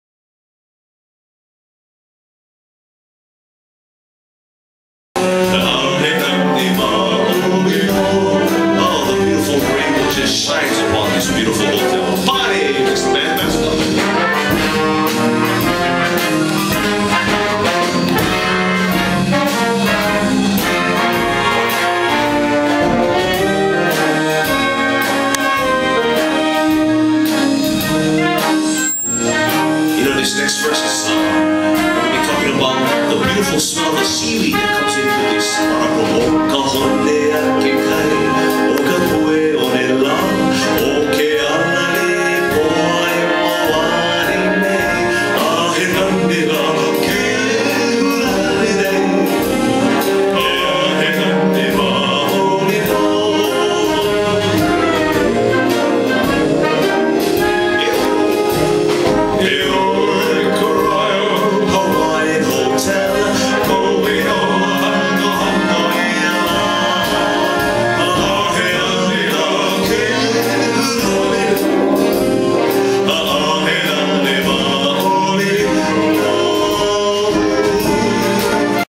The Royal Hawaiian Band and their Beautiful Hula Dancer
Beautiful music, beautiful dancing.